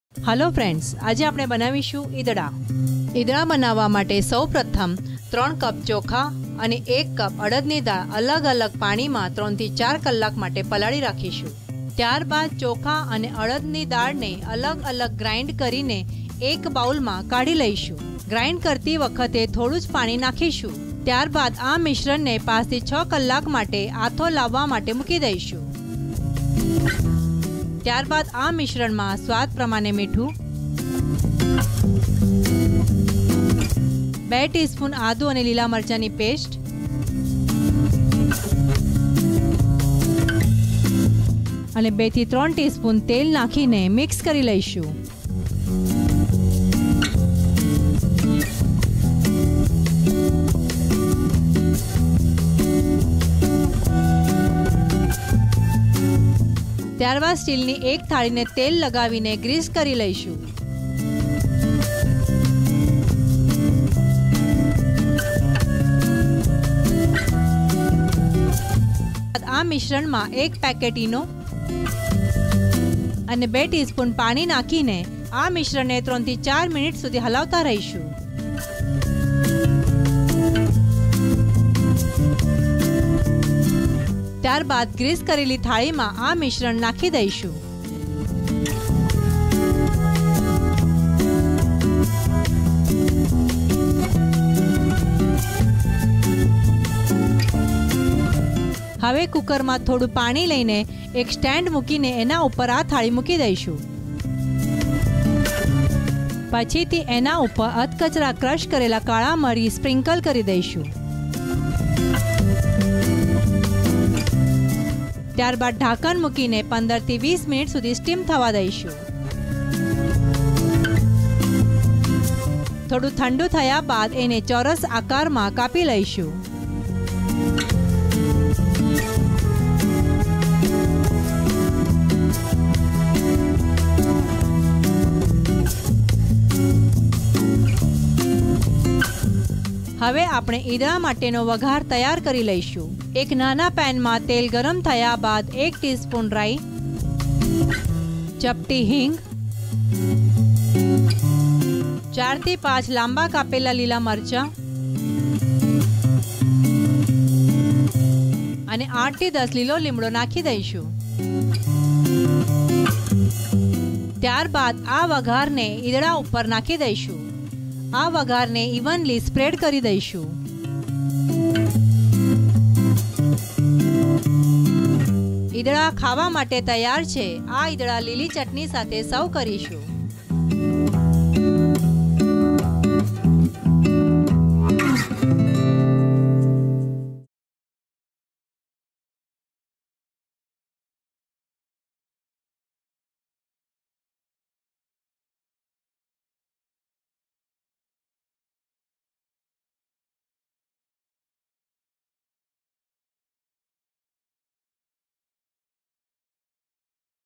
Idada Recipe - ઇદડા | Recipes In Gujarati [ Gujarati Language] | Gujarati Rasoi
Idada is a popular Farsan recipe and is also known as white dhokla sometimes. This whit dhokla recipe or Idada recipe is very easy to make and this Gujarati farsan recipe is enjoyed by everyone. Idada is enjoyed with Keri No Ras by dipping the fluffy Idada into sweet keri no ras. Idada or White Dhokla recipe is a very popular recipe in the summer season.

Recipe:

Ingredients –
1 Cup Urad Dal
3 Cup Rice
2 Teaspoon Ginger-Green Chilli Paste
2-3 Teaspoon Oil
1 Eno Sachet
Crushed Black Pepper
1 Teaspoon Mustard Seeds
Pinch of Hing
4-5 Split Green Chillies
8-10 Curry Leaves
Water
Salt

Method –
1. Soak Urad Dal and Rice for 3-4 hours separately.
2. Grind them separately and prepare a batter.
3. Let the batter ferment for 5-6 hours.
4. Add salt as per taste.
5. Add ginger-green chilli paste and some oil. Mix well.
6. Grease a steel plate with oil.
7. Add eno to the mixture and some water and mix well.
8. Now add this mixture in the greased plate.
9. Place the plate in the cooker to steam.
10. Sprinkle some crushed black pepper and let it steam for 15-20 minutes.
11. Remove it and let it cool.
12. Then cut it into square pieces.
13. For the tadka heat some oil, add mustard seeds, hing, green chillies and curry leaves.
14. Add this tadka to the Idada.
15. Idada is ready to eat.